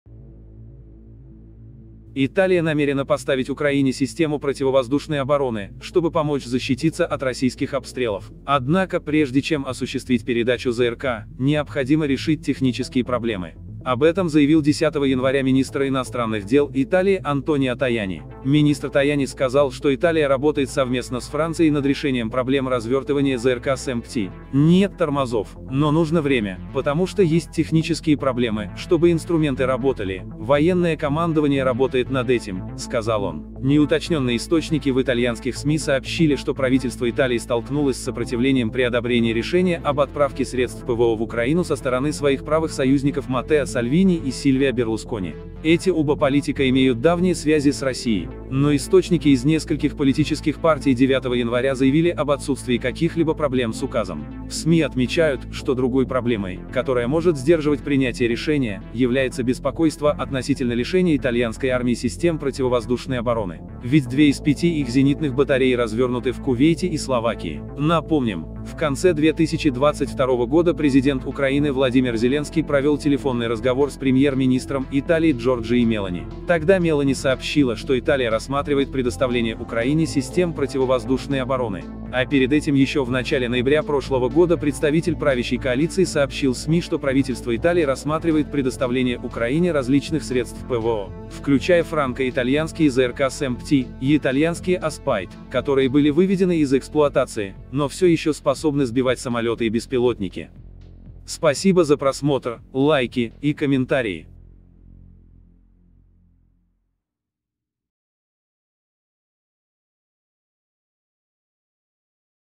Италия намерена поставить Украине систему ПВО SAMP/T
Италия намерена поставить Украине систему ПВО/ПРО, чтобы помочь защититься от российских обстрелов